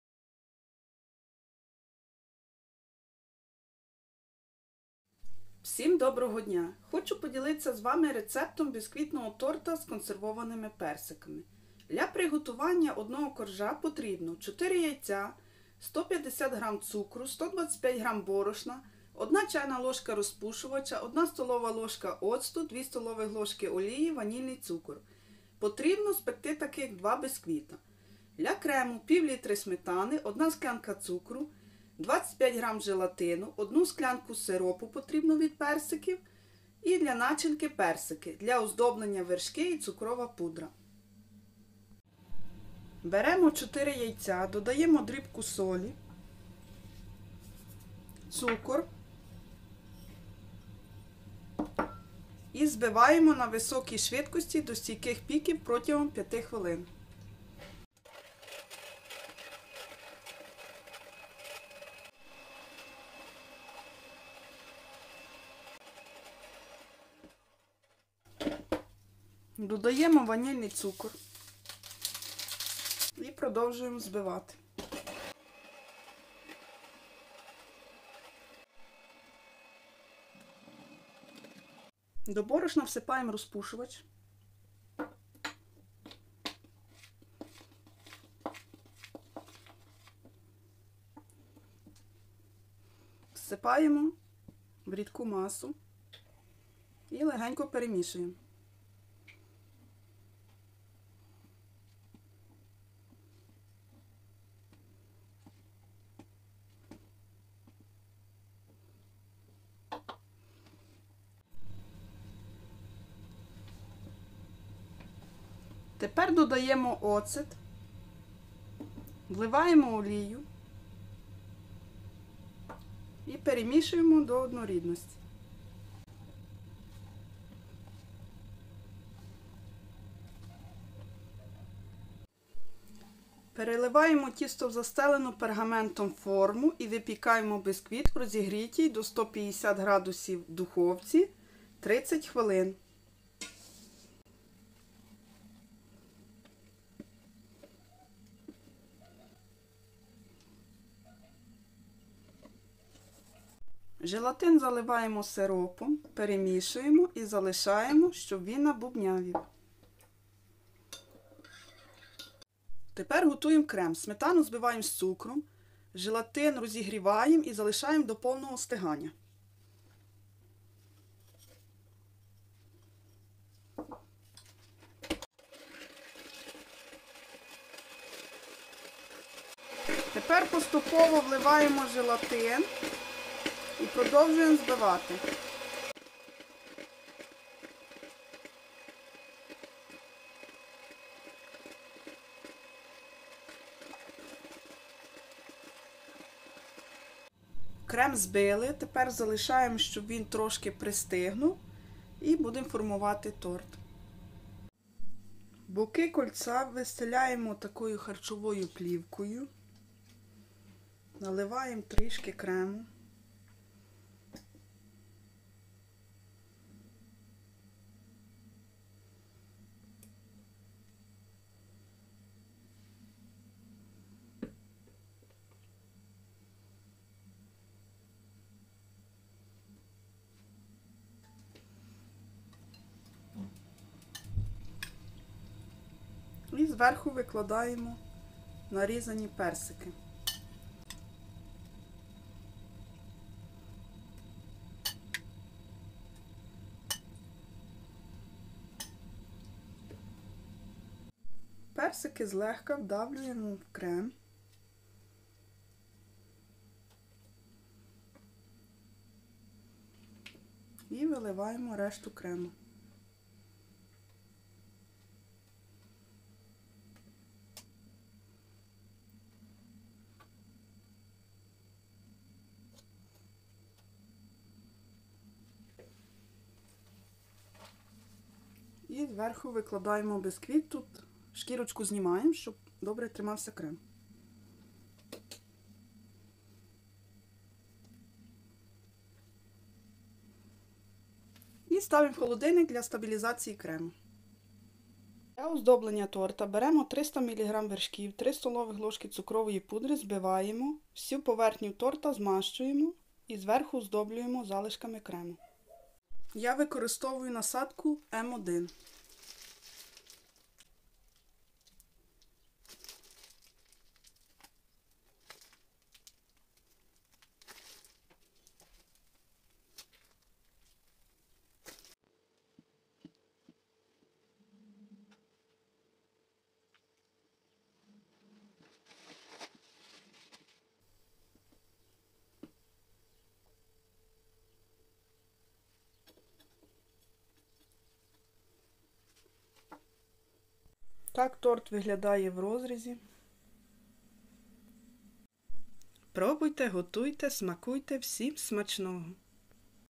Бісквітний торт із консервованими персиками.
Рецепт:
Для одного коржа:
яйця-4шт.
цукор-150 гр.
оцет-1 ст.л.
олія-2 ст.л.
борошно-125 гр.
розпушувач-1ч.л.
ванільний цукор-1 п.
Спекти таких два коржі.
Для крему:
сметана-0,5л.
цукор-1скл (склянка 250 мл).
сироп з персиків-1 скл.
желатин-25 гр.
Для начинки:
1л.банка персиків.
Для оздоблення:
вершки-300мл.
цукрова пудра-3 ст.л.
Випікати при 150 C-30 хвилин.
Смачного.🌼🌼🌼